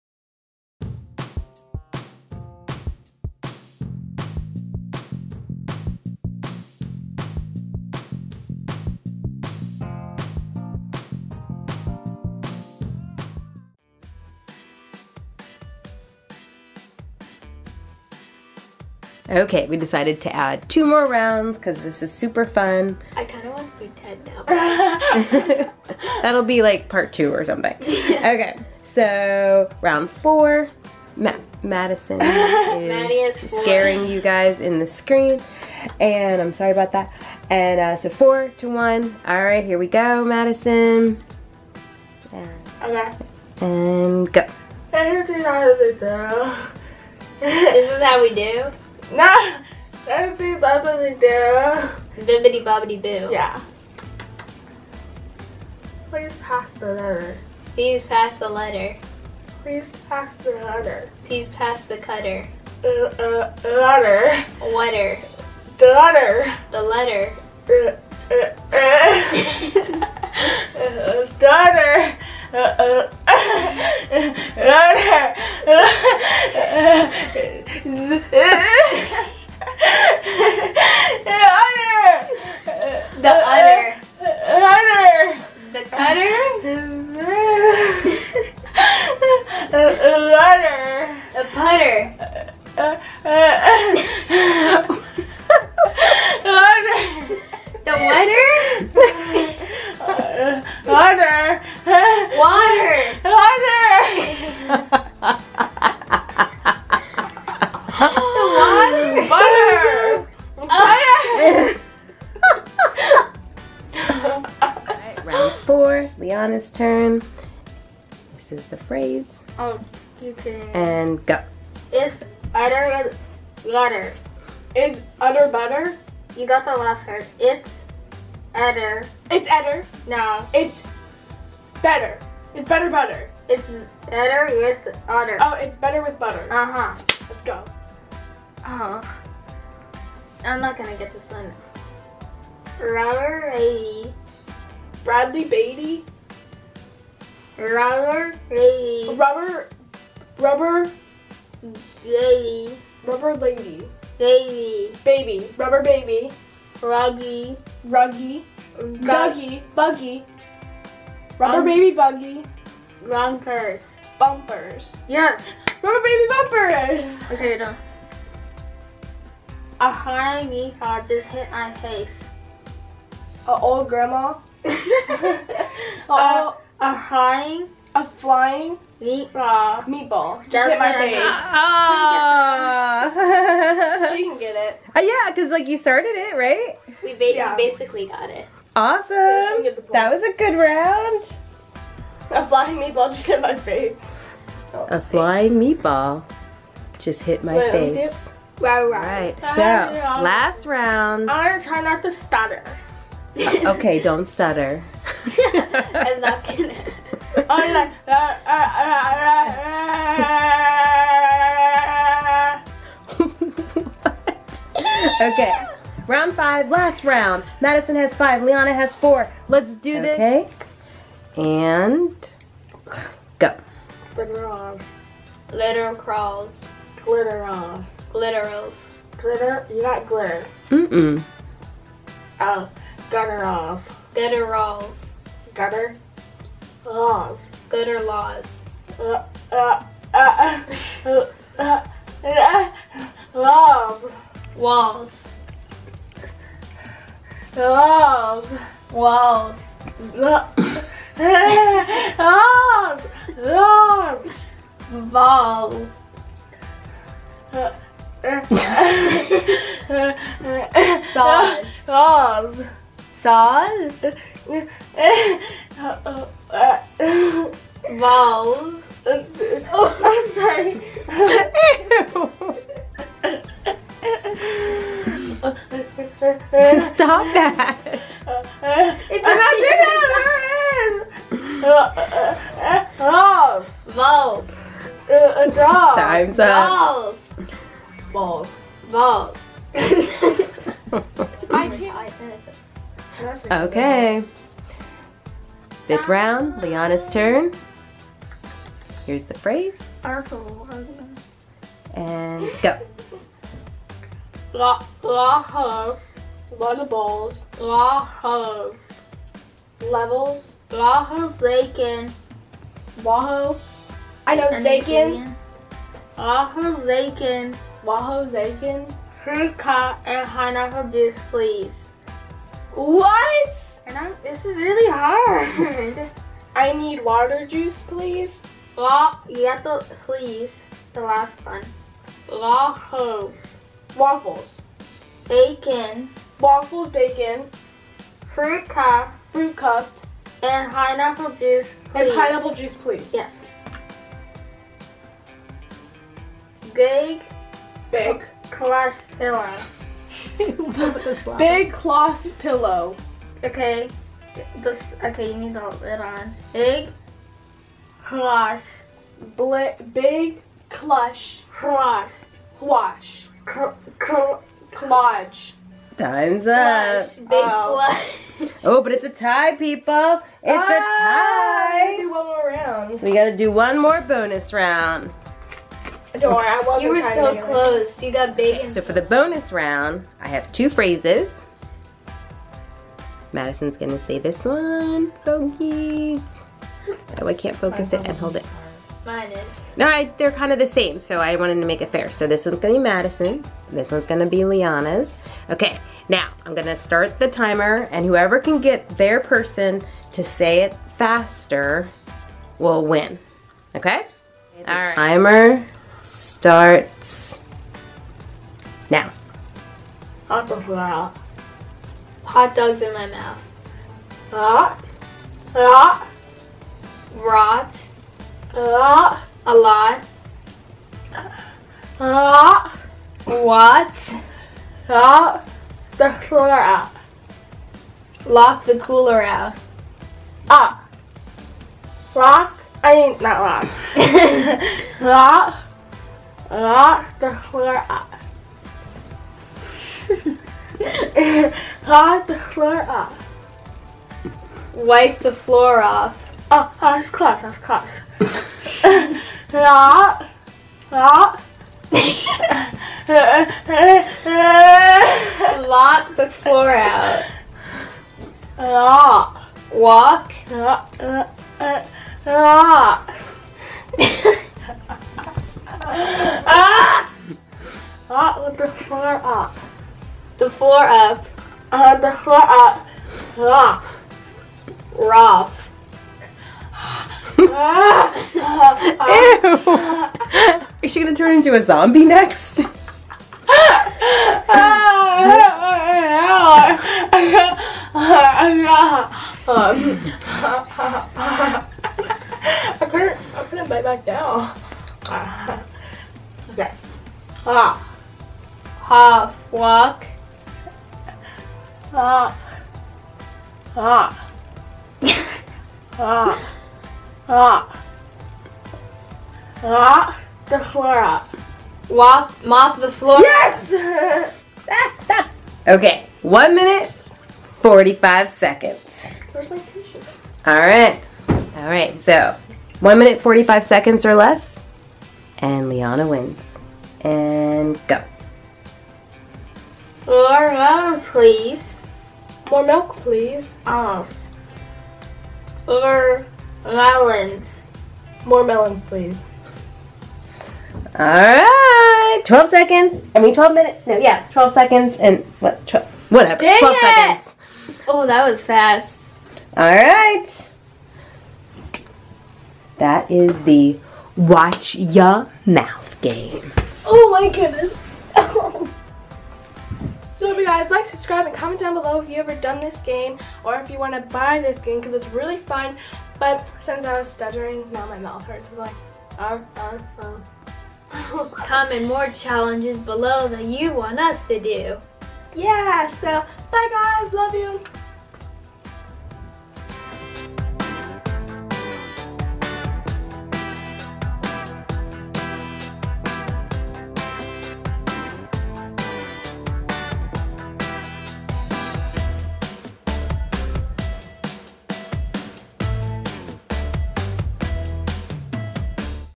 Watch Ya Mouth Game - Part 2 | Mouthguard Challenge | In Mad's World
PART 2 of the Watch Ya Mouth game - it's hilarious! Watch Madison and Liana try and play this mouth guard game. ♡ Love this video?
🤔How do I turn on video notifications?🤔